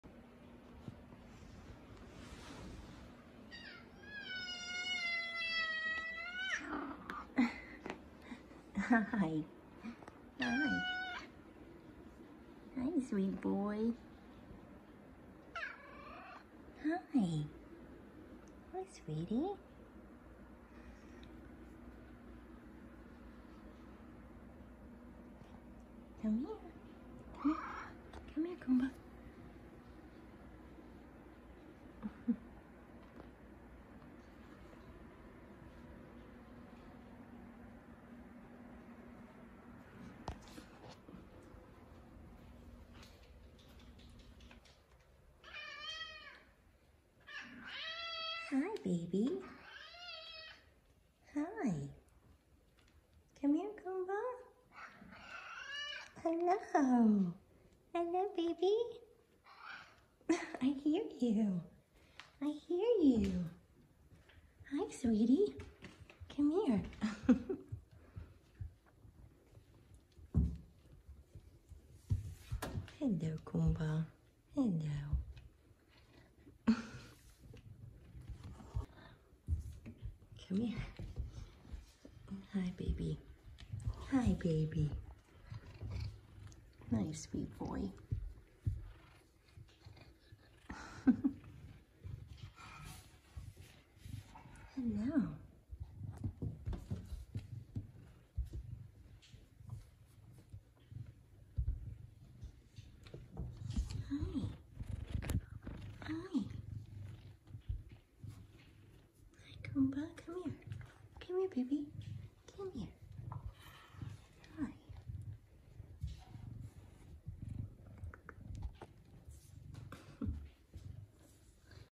Big Savannah Cat Kumba Meowing And Being Lovable!
Big Cat Meowing// F2 Savannah cat Kumba being his sweet self meowing and giving lovins! He’s my gentle giant! 😻